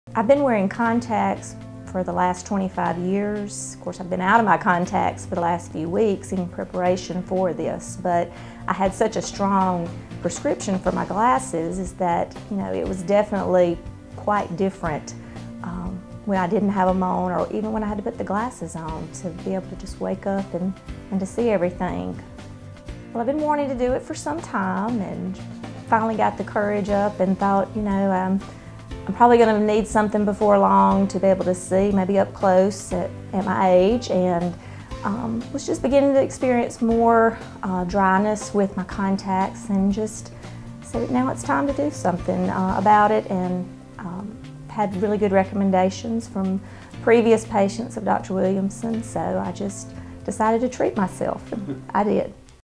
Sandi Flynt says iLASIK MEANS NO CONTACTS!
Sandi Flynt came to Hattiesburg Eye Clinic because of the eye surgeons reputation for quality care. After iLASIK she can now see without contacts or glasses. Hattiesburg Eye Clinic was the first to provide patients blade-free iLASIK, the most experienced team the most advanced technology!